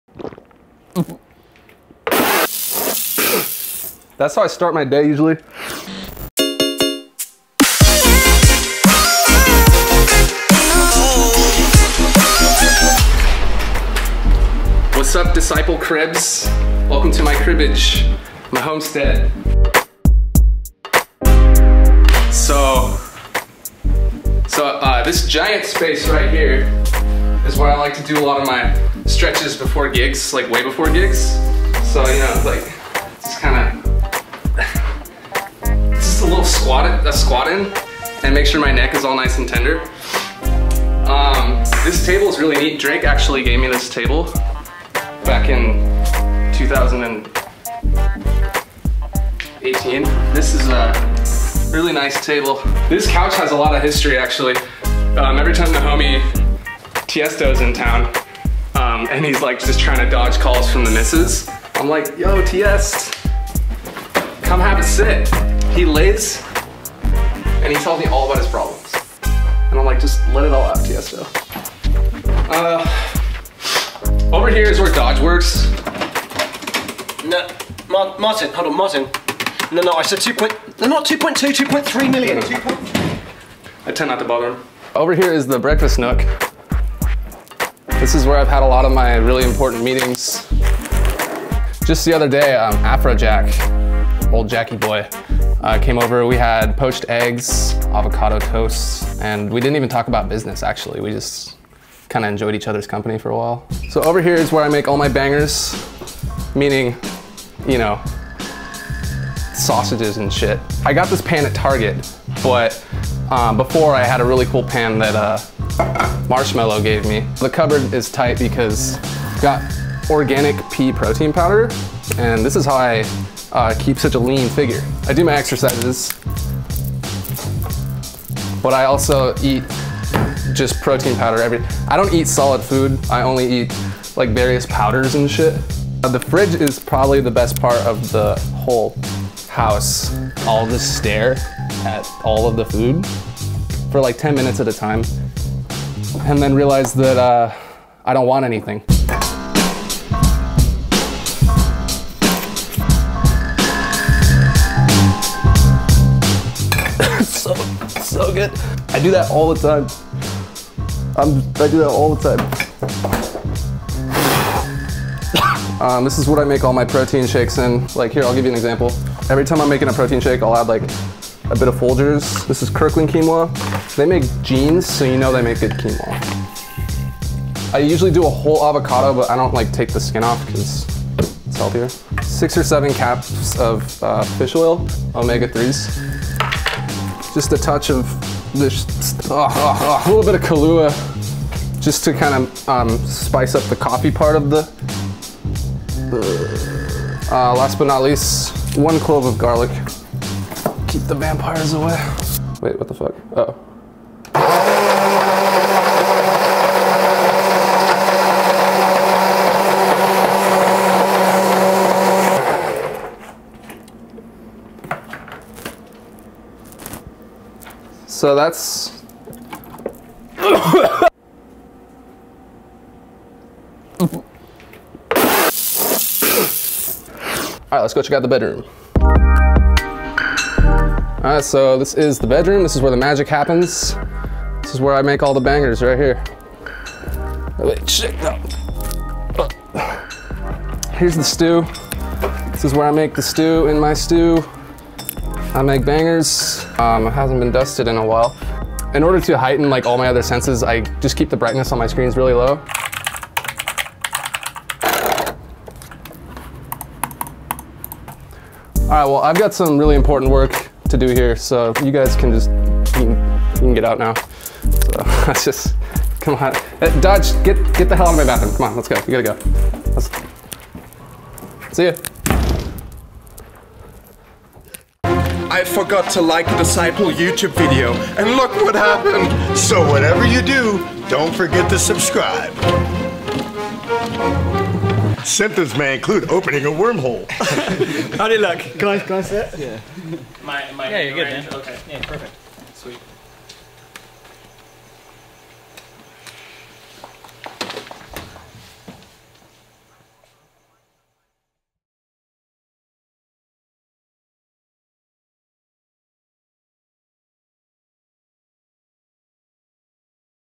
Disciple Cribs: Eliminate
Welcome to Disciple Cribs: Eliminate Edition! Get a glimpse into the life of Nate and marvel at his couch, his blender, and more!